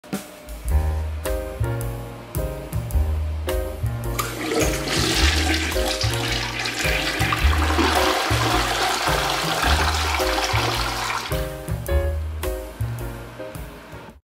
You can get rid of poop and urine by flushing the toilet.
It's hard to survive in this Isekai, but let's all do our best.

How to flush a toilet